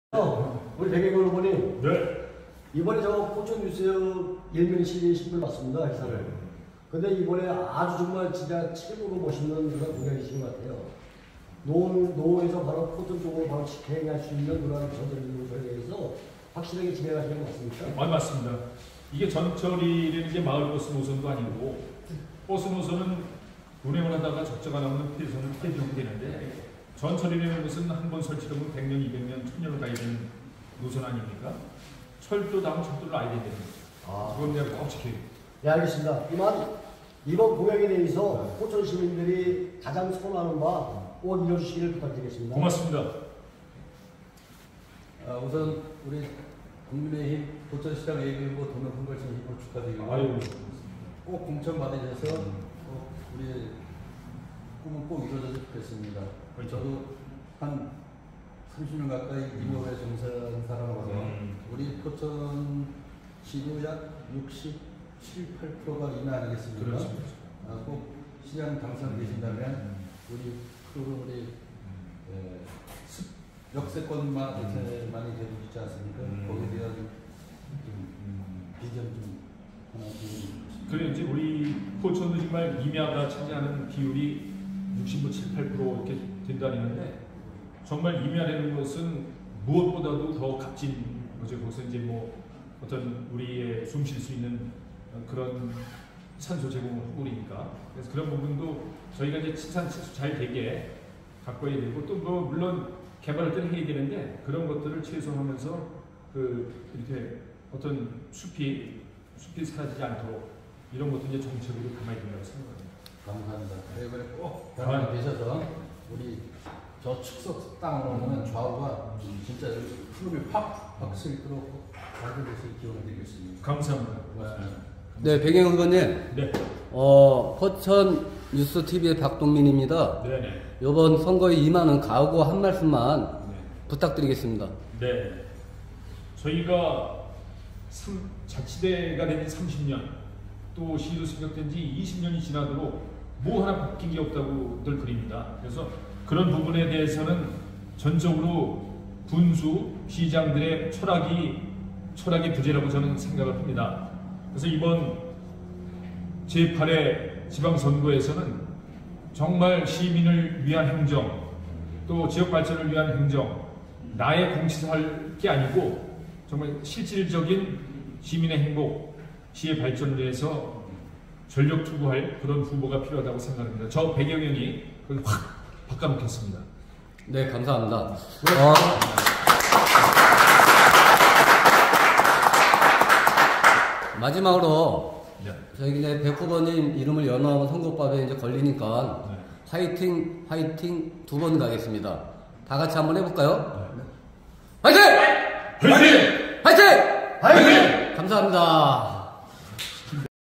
백영현에게 묻는다/박동민 포천뉴스TV/짤 인터뷰/직캠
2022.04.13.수
백영현 선거사무소에서

촬영편집:박동민